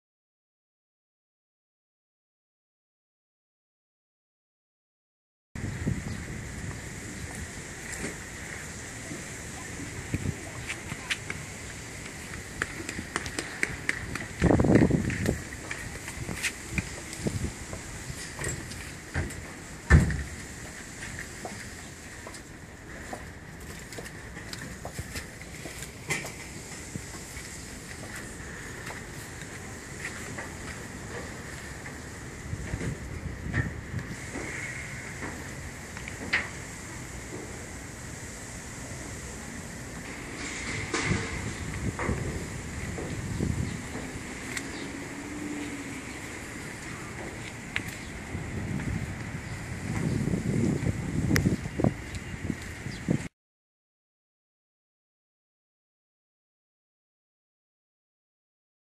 Type 59/63 North Korea/Chinese Tanks
Type 59 MBT and Type 63 Amphibious Tank, located in Seoul, Korea. These are two types of tanks created by China and currently in use by North Korea.